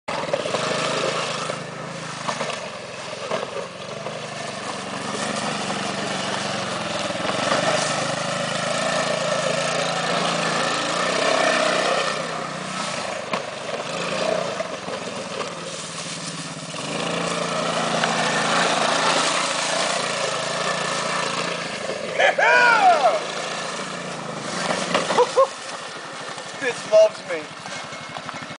Andy on 2 Wheels pt 1
Andy on his 4 wheeler @ the lake